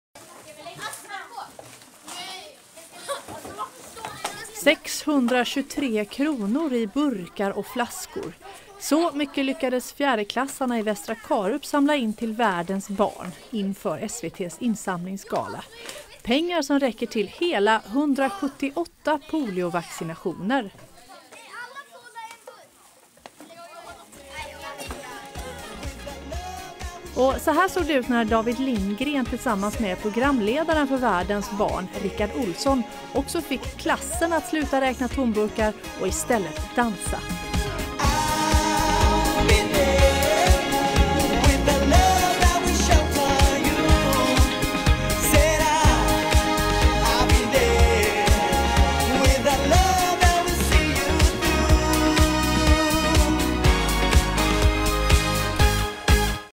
Burkar och dans mot polio
Över hundra vaccinationer mot virussjukdomen polio lyckades fjärdeklassarna i Västra Karup, få ihop till genom att samla burkar och flaskor till Världens barn.

Publicerad på hd.se måndag 6 oktober 2014.